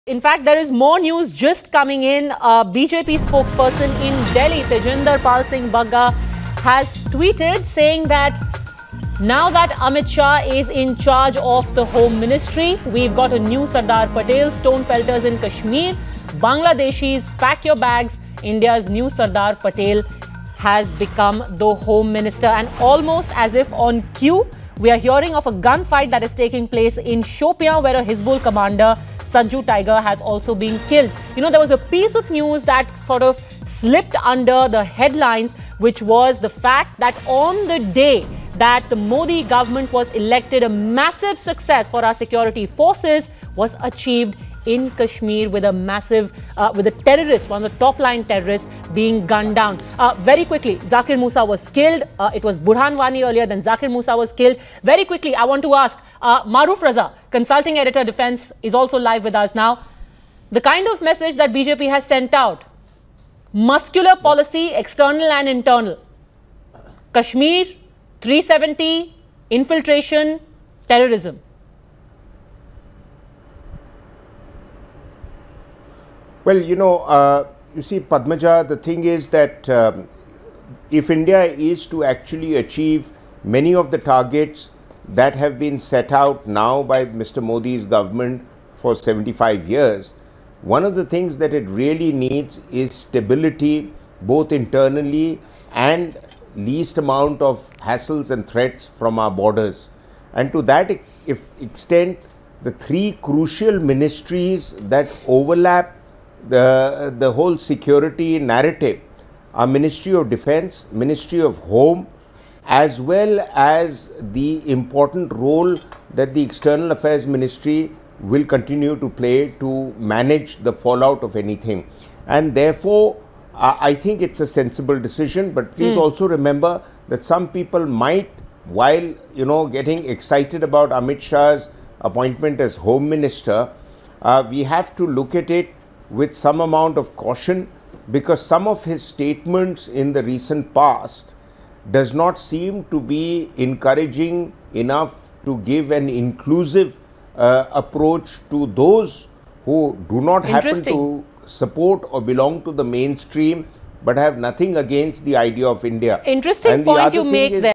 Delhi BJP spokesperson Tajinder Pal Singh Bagga tweets and WARNS stone pelters & inlfiltrators
BJP spokesperson Tajinder Singh Bagga has now tweeted and warned the stone pelters and infiltrators. He tweeted, 'Stone pelters in Kashmir, Bangladeshi Pack your Bag. India Got New Sardar Patel as Home Minister Amit Shah'. Watch to know more!